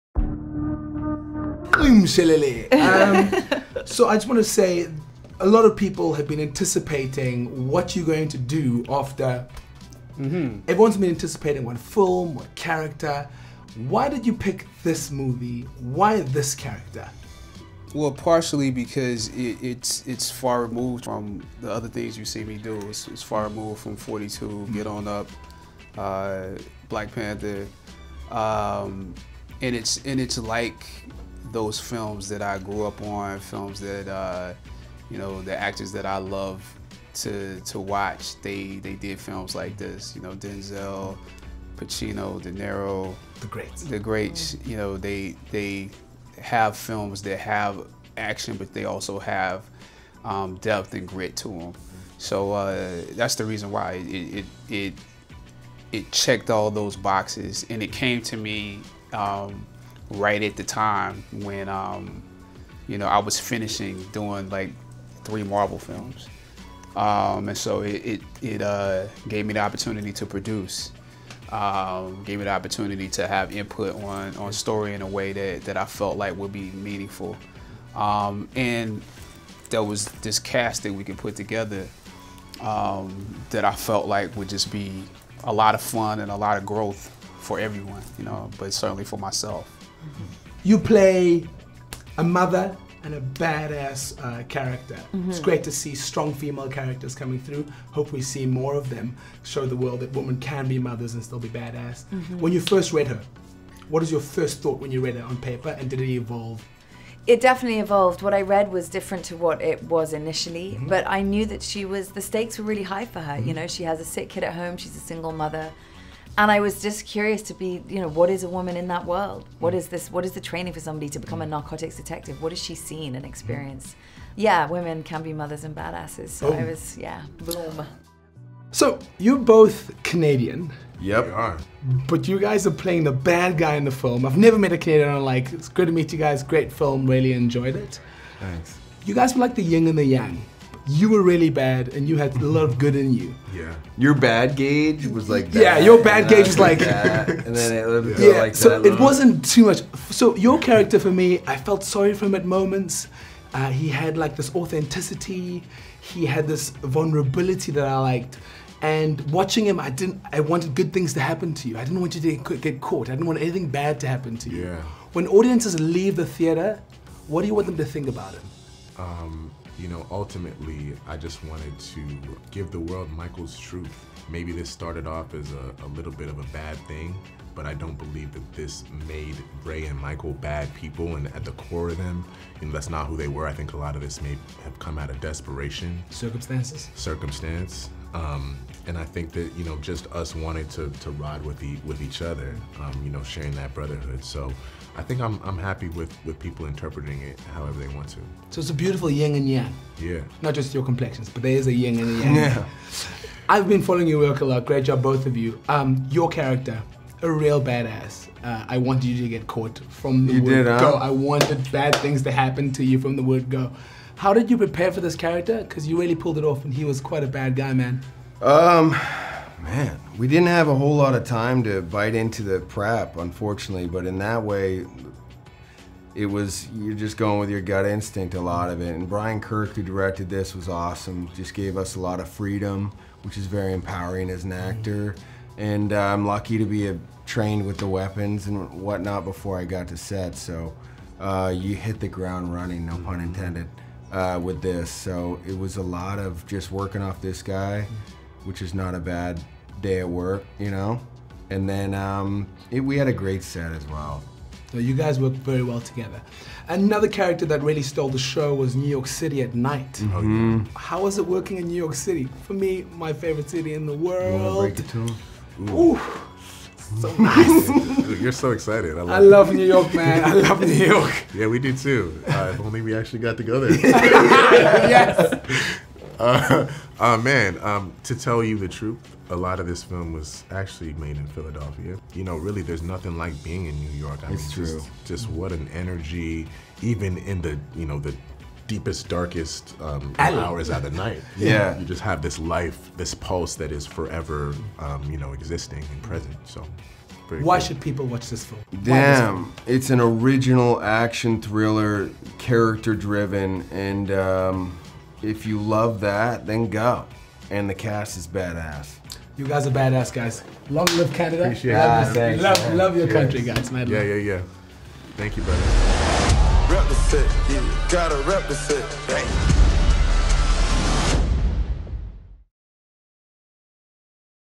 Siv Ngesi brings exclusive 21Bridges inside scoop with the CAST!
The stars lined up to chat everything 21Bridges with Siv Ngesi. Non-stop action, plot-twists to make your head spin, and a ridiculously good-looking cast that includes Chadwick Boseman, Sienna Miller, Stephan James, and Taylor Kitsch? Better strap in for this one!